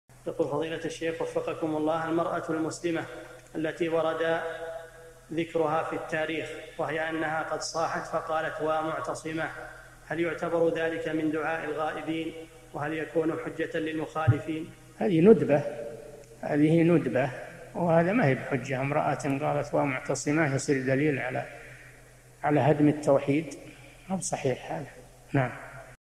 [177 /1350] المرأة المسلمة التي صاحت "وامعتصماه "! هل يعتبر ذلك من دعاء الغائبين؟ صالح الفوزان
[177 /1350] السؤال: المرأة المسلمة التي ورد ذكرها في التاريخ، وهي أنها قد صاحت فقالت: "وامعتصماه "! فهل يعتبر ذلك من دعاء الغائبين وهل يعتبر حجةً للمخالفين؟
 الجواب: هذه ندبة، وهذا ما هي بحجة امرأة قالت: "وامعتصماه" يصير دليل على هدم التوحيد؟! ما هو بصحيح.
للاشتراك في قناة الشيخ صالح الفوزان
أسئلة وأجوبة من شرح قاعدة جليلة ( 1350 ) مقطع

للاستفسار والإقتراحات والدعم
رئيس الفريق
لمشروع كبار العلماء